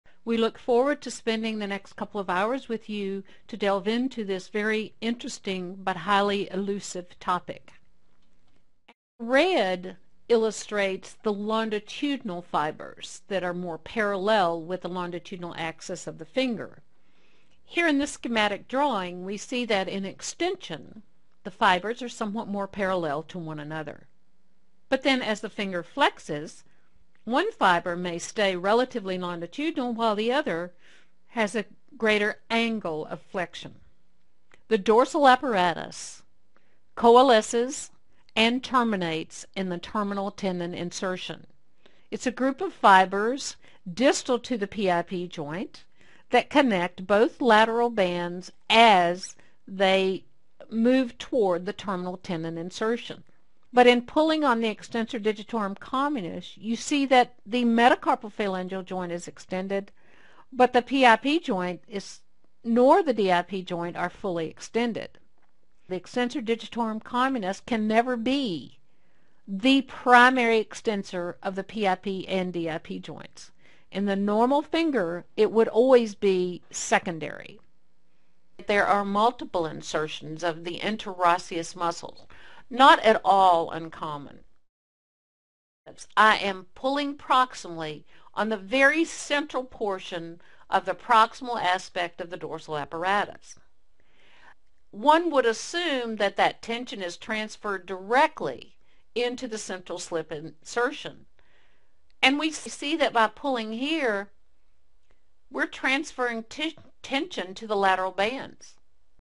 Understanding the Dorsal Apparatus of the Finger Preview
This is a video preview for one of many online courses for continuing education credit we offer

This two hour lecture unravels the puzzle of the dorsal apparatus of the finger. Understanding the complex structures within the dorsal apparatus allows you to comprehend the intricate movement balance of the finger.  Using custom professional anatomical illustrations, the anatomy comes alive, giving you an in-depth appreciation of how this anatomy works. This course is the first part of six lectures that are intended to be viewed in sequence.